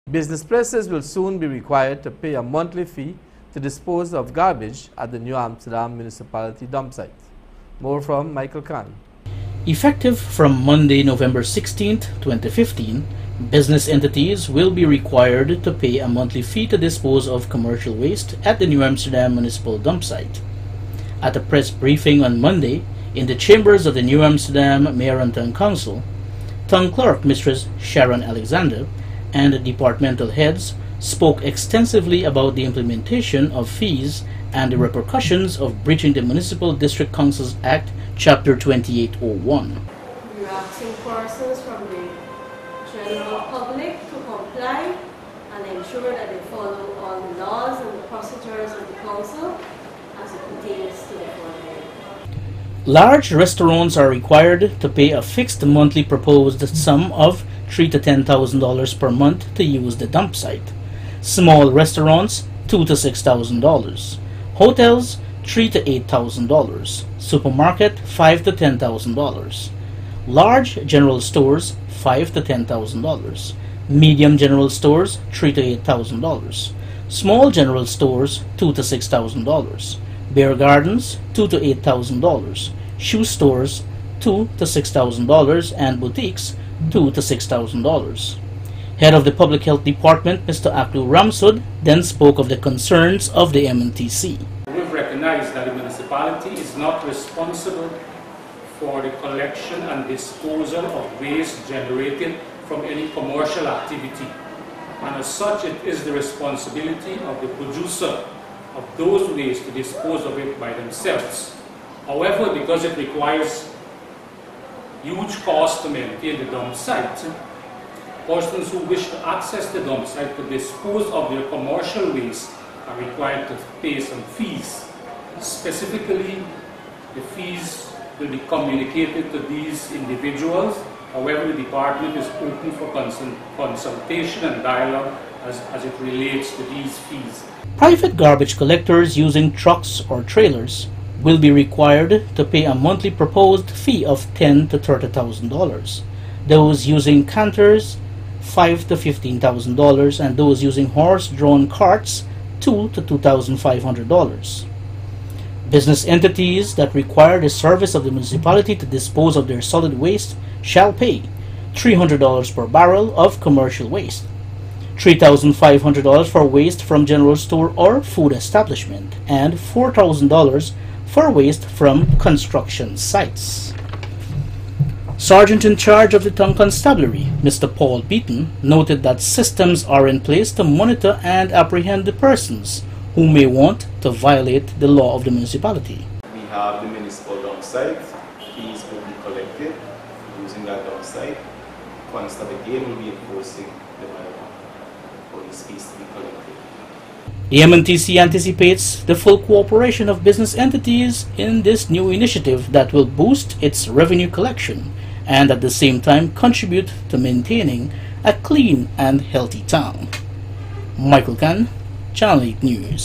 Monthly Fees To Enforced For Use of N/A Municipal Dump
Business Places will soon be required to pay a monthly fee to dispose of garbage at the N/A Municipality Dumpsite.